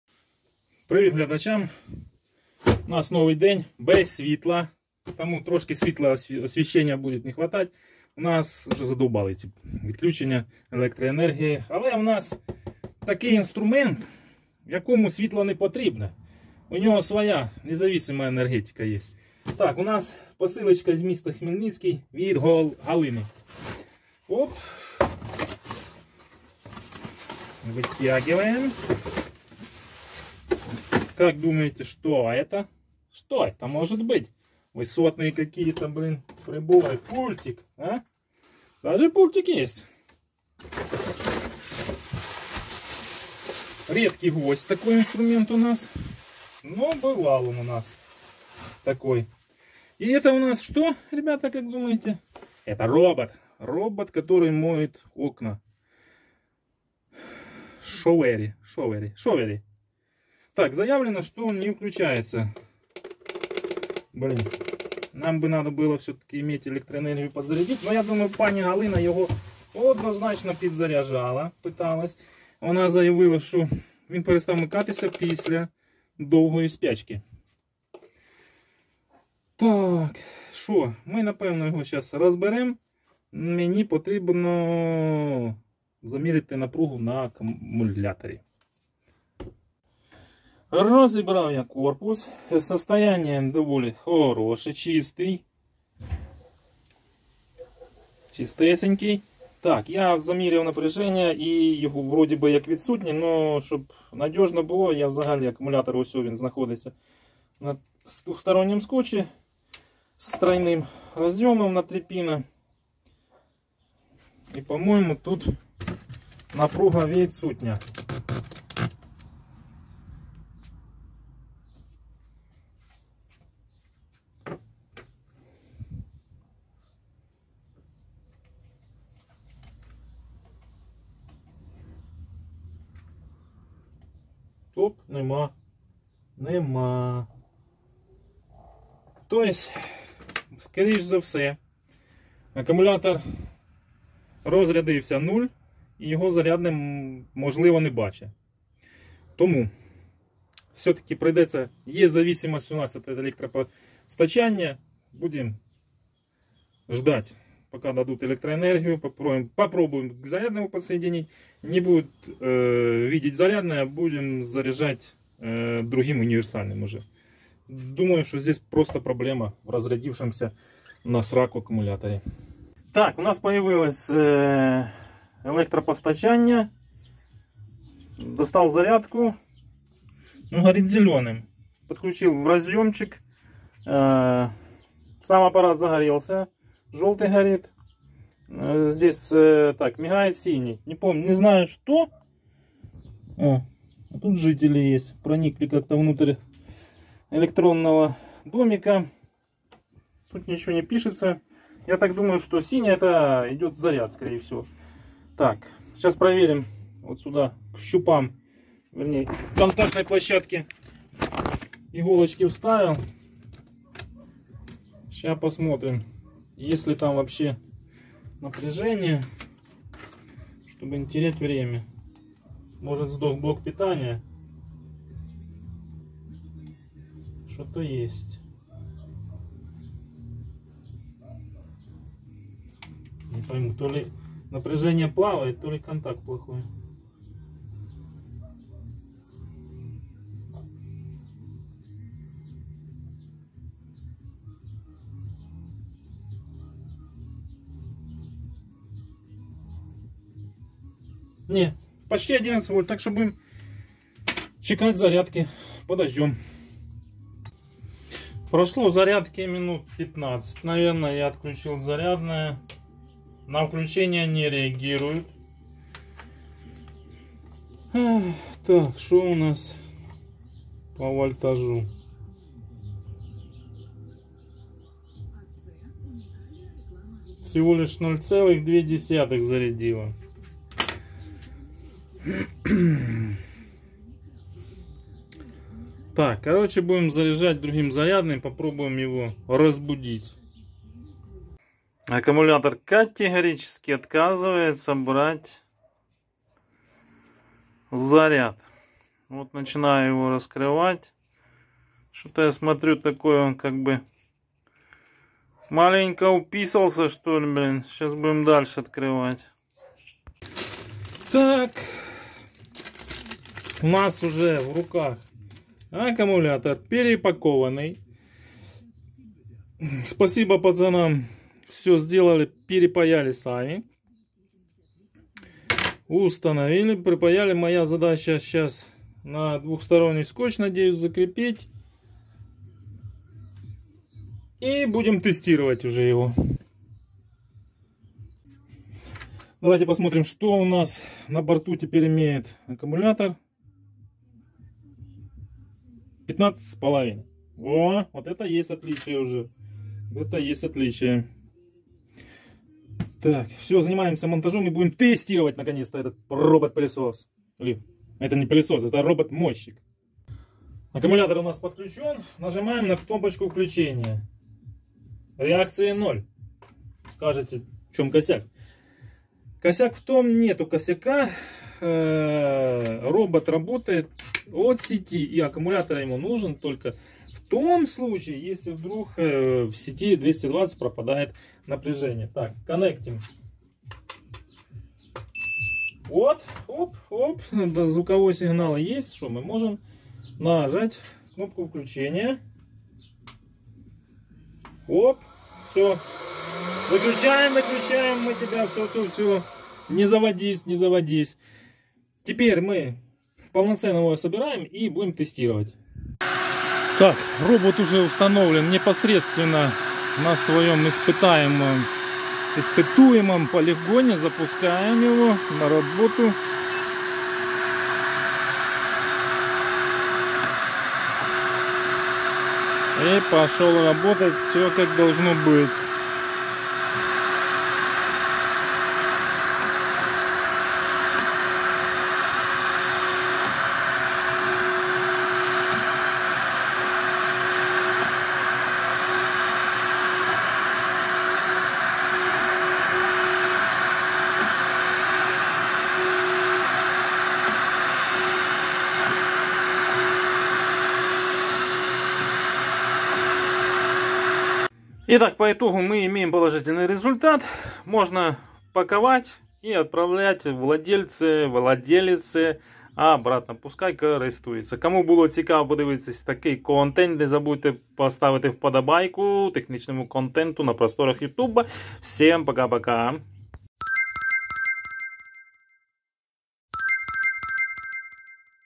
Робот мойщик окон CHOVERY не включается - ремонт.
Робот для мытья окон Chovery CL.1 после долгого простоя не включается: разборка, замена акб.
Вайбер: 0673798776